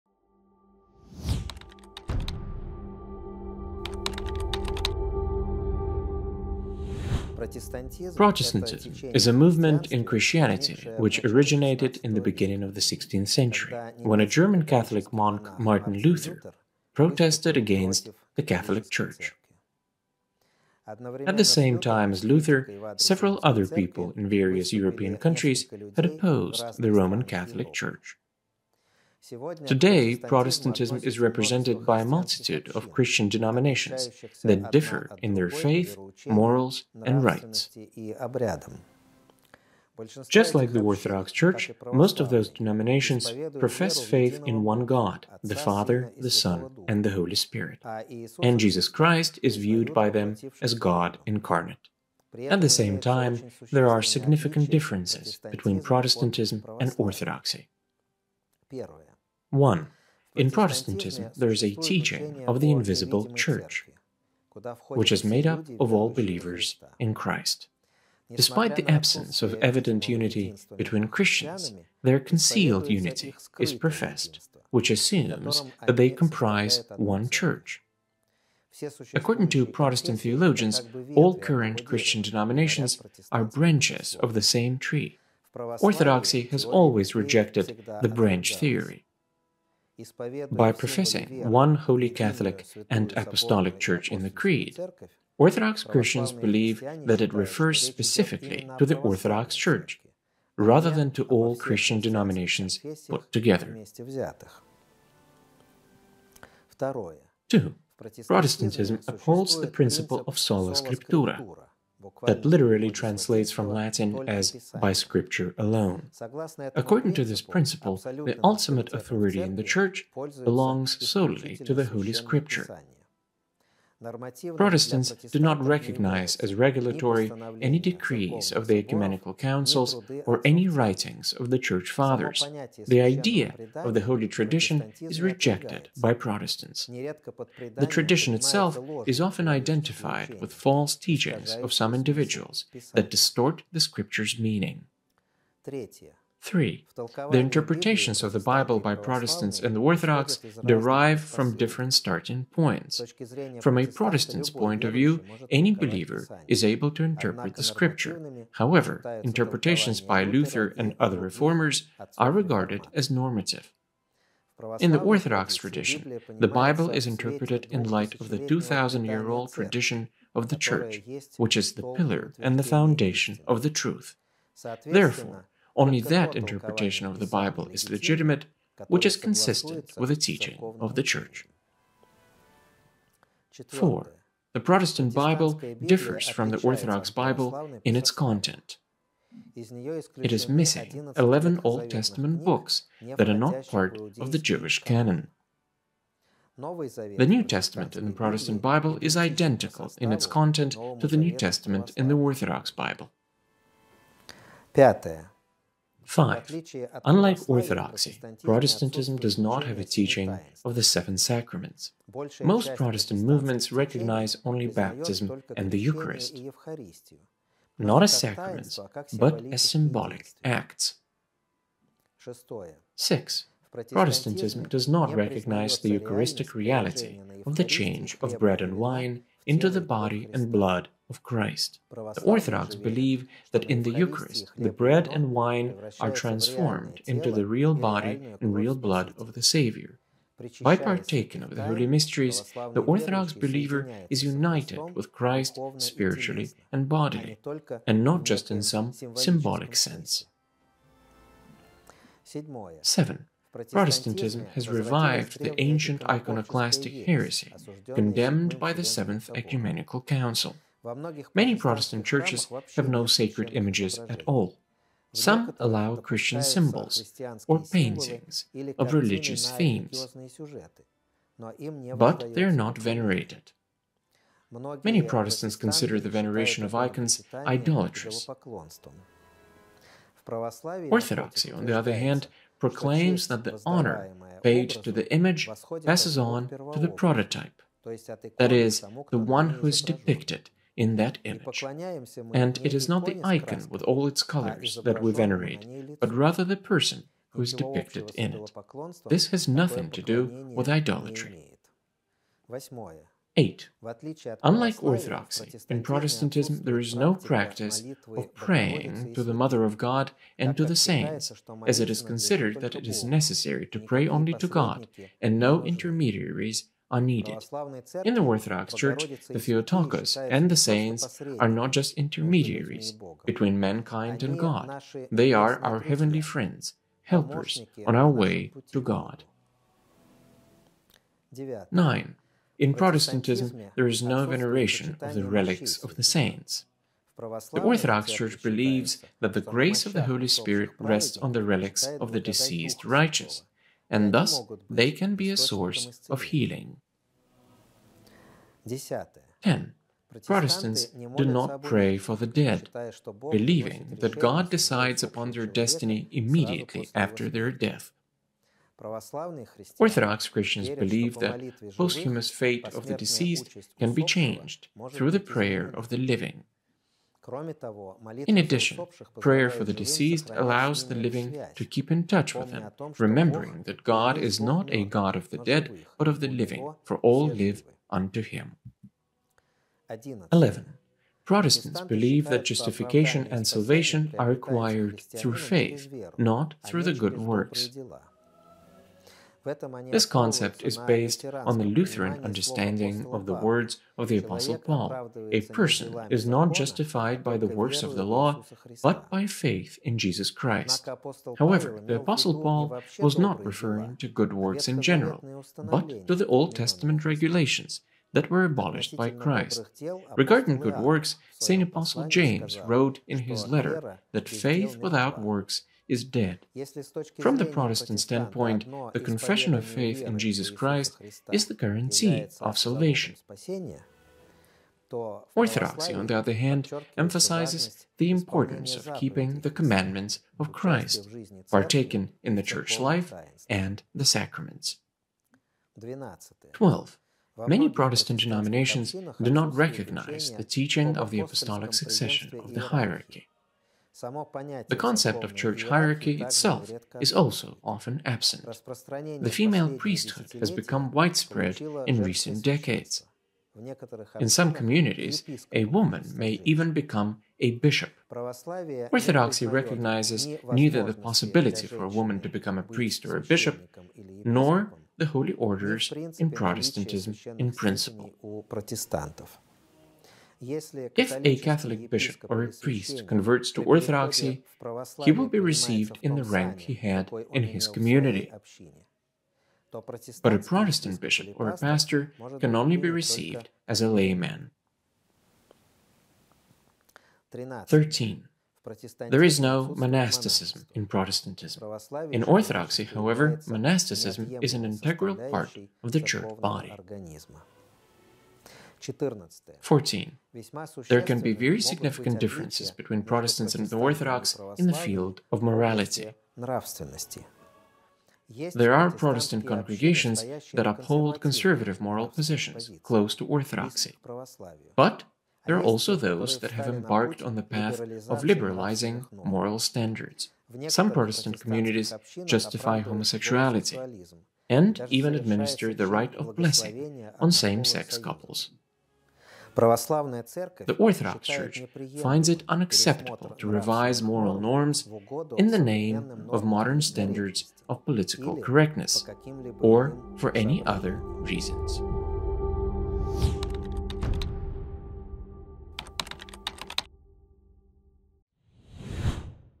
Orthodoxy and Protestantism: 14 DIFFERENCES. Essentials: in Brief.
Essentials: in Brief - a catechetical programme of Metropolitan Hilarion of Budapest and Hungary.